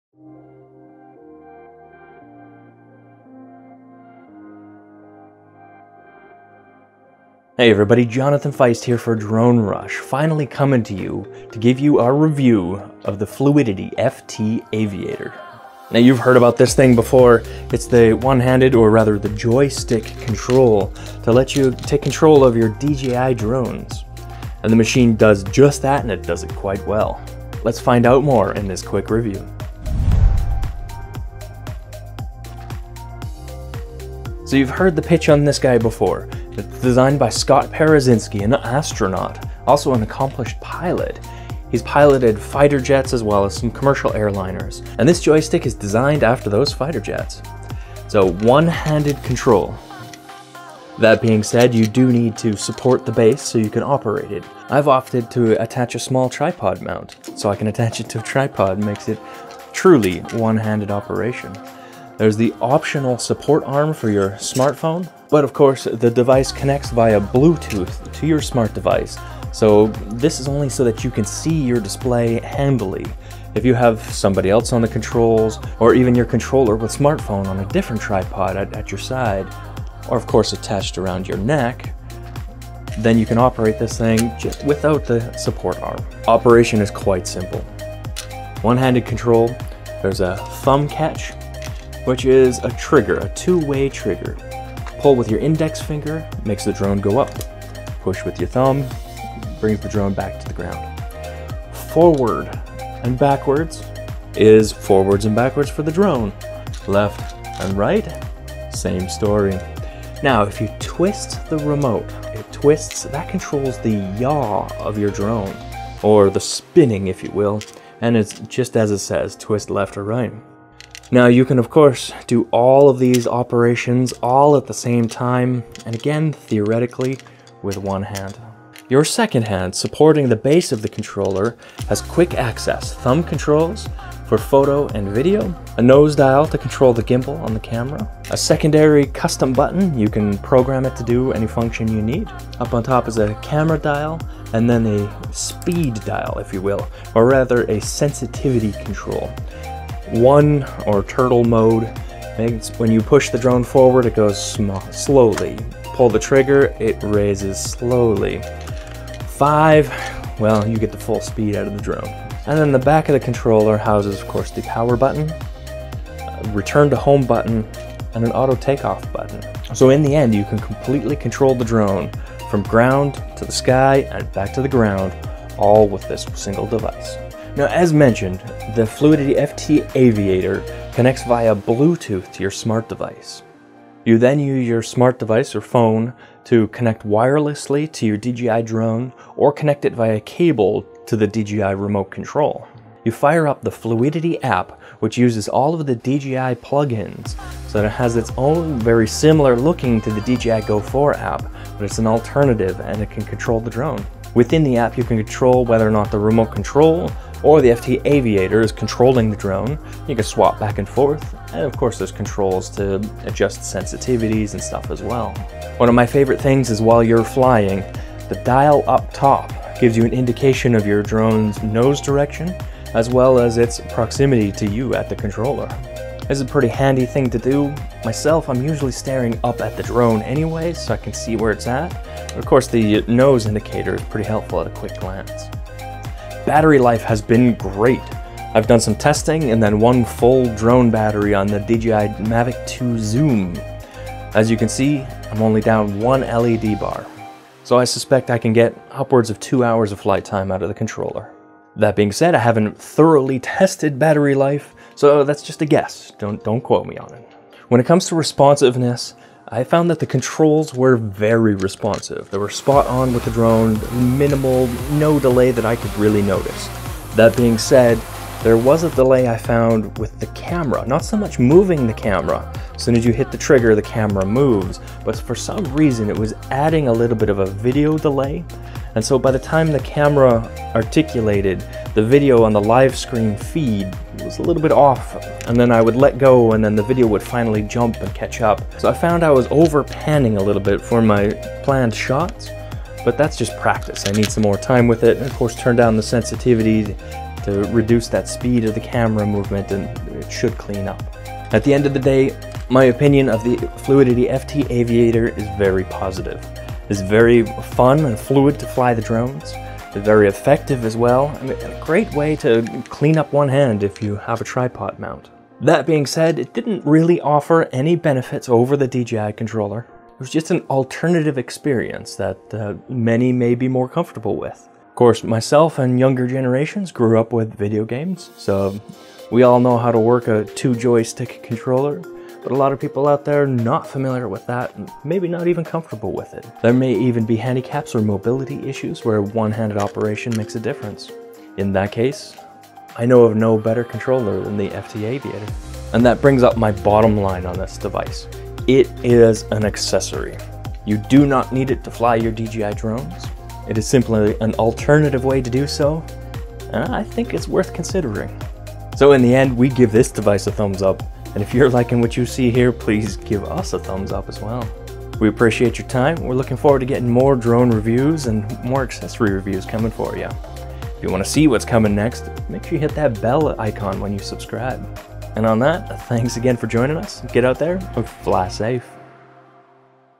Fluidity FT Aviator Review: Cool drone joystick controller!
The Fluidity FT Aviator lives up to the expectations! We called this the best drone accessory at CES 2019, and now that we've been able to test it, we stand by that claim.

Joystick operation for your favorite DJI drone is fun, fluid and stable.